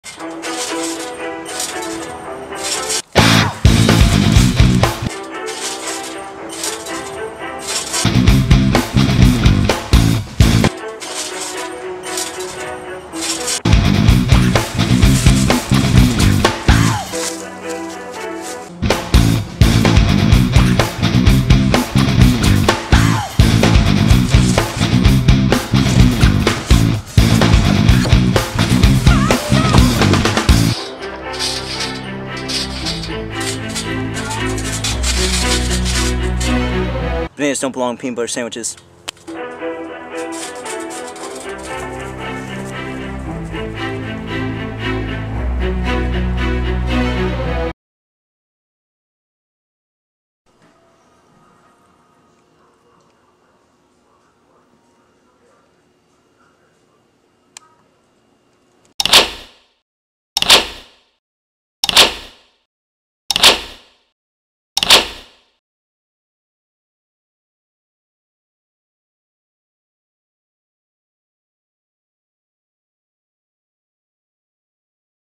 Banana sandwich
Part one of the angry banana series.
Just when you think that its safe to eat a peanut butter and banana sandwich, the yellow avenger strikes!!